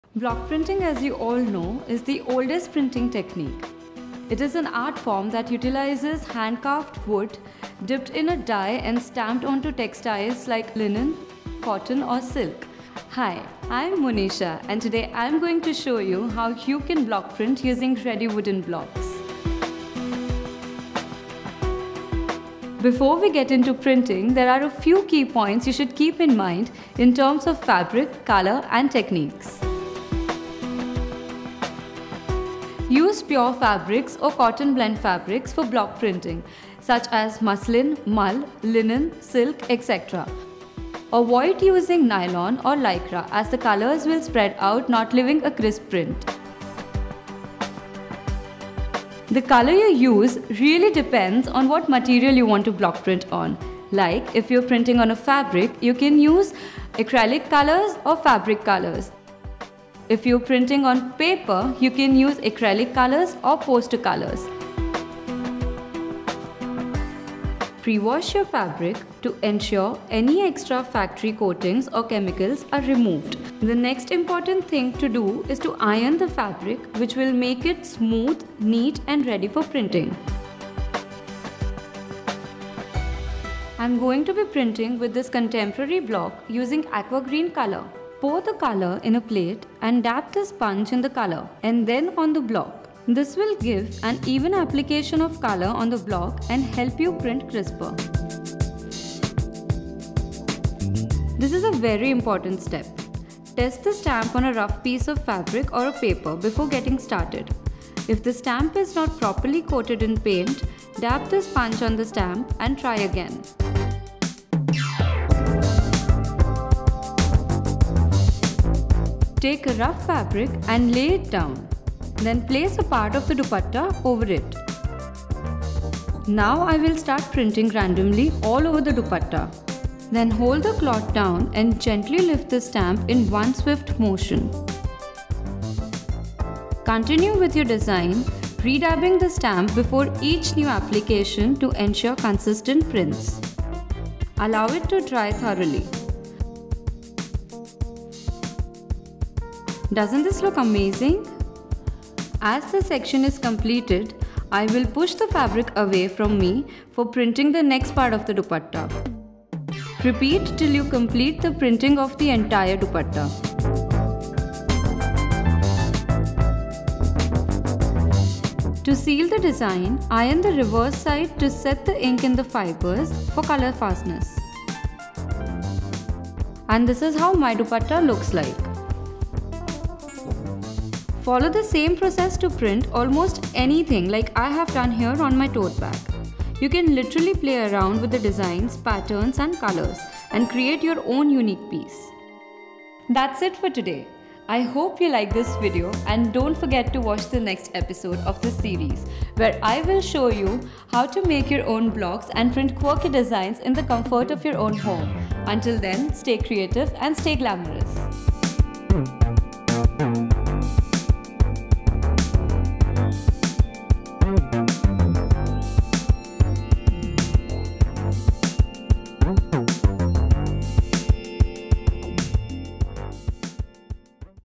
DIY: Block Printing At Home | Fabric Printing Technique
Block printing is an oldest printing technique, which has been around for over thousand years. Loaded with colours, patterns and creativity, this art is done with help of wooden blocks dipped in color trays and printed on fabrics.

In this video Monisa show you DIY: Block Printing At Home | Fabric Printing Technique